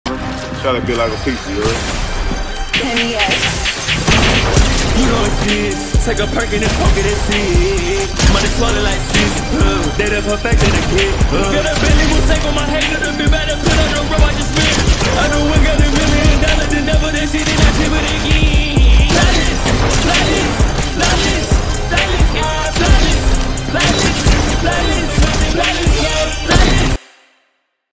flawless - for a friend 🤝
felt like i needed to post something to feel better from stress and feeling down, so here's an unreleased montage from a few months ago I had made for a friend. VERY simple one, but just like he hoped for it to be. 💓